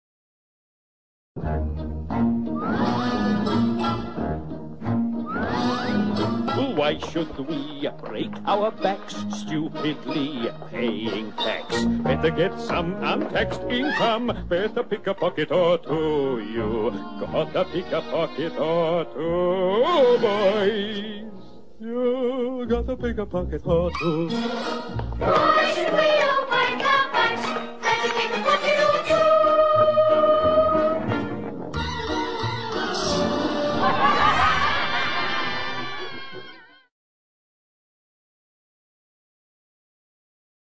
Funny Parliament Song
A funny song about the house of commons.